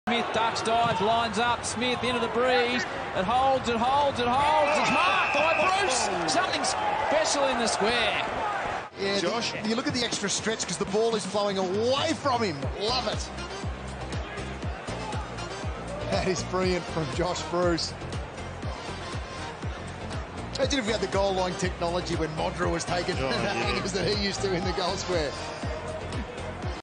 Josh Bruce Mark Of The Year Contender - Bulldogs Vs Adelaide Round 20
Sit back and enjoy Josh Bruce’s spectacular mark in the goal square. Do you think this will win mark of the year?